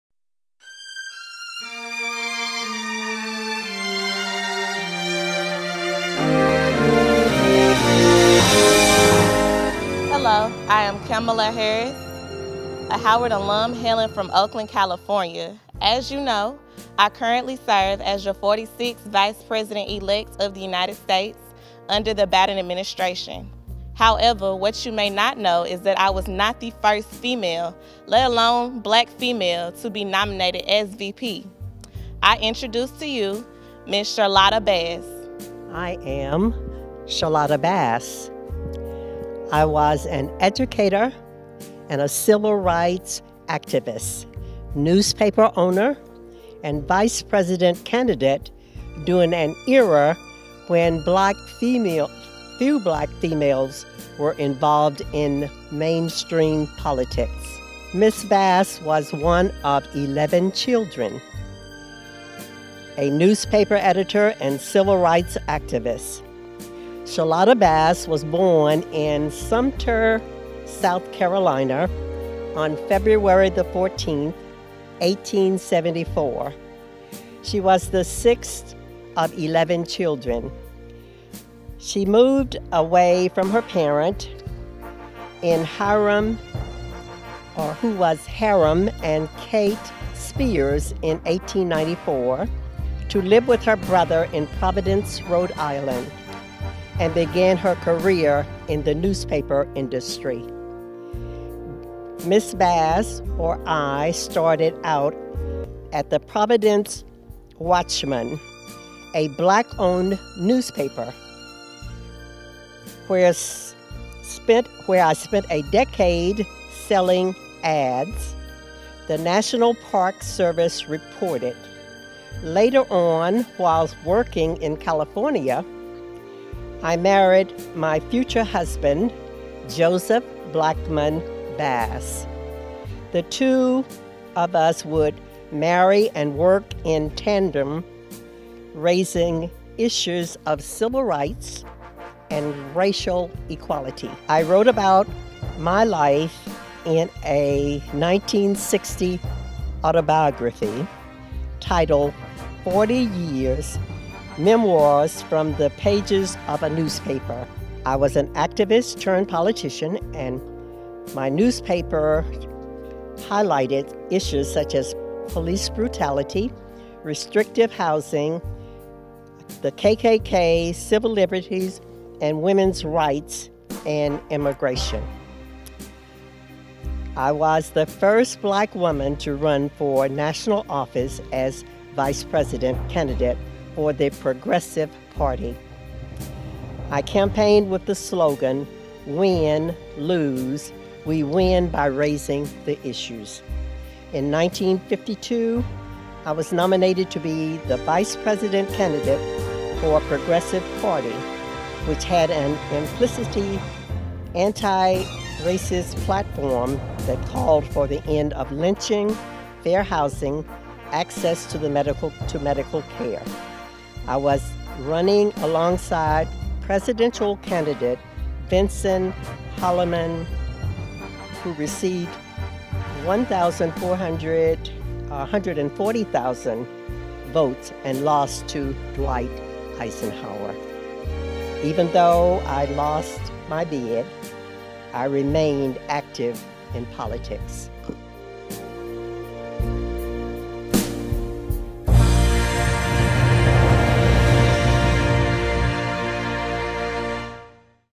2021 Kamala Harris Charlotta Bass | Living History Museum - Hidden Figures of African American Hist.
Black History Month is a time to recognize African American achievements, and inspire everyone to believe in the power of a dream. The Larry A. Thompson Ministry at Lake Providence Missionary Baptist Church presents a virtual Living History Museum - “Hidden Figures in African American History”,  which debuted on Sunday, February 14, 2021 on all Lake Providence Missionary Baptist Church social media platforms.

We acknowledge the important role African Americans have played in our nation’s history and their many contributions in everything from the arts to politics to inventions and scientific advancements. Our prayer is that this information about the rich history of African Americans will inspire everyone to pursue their dreams. Today we honor Kamala Harris and Charlotta Bass.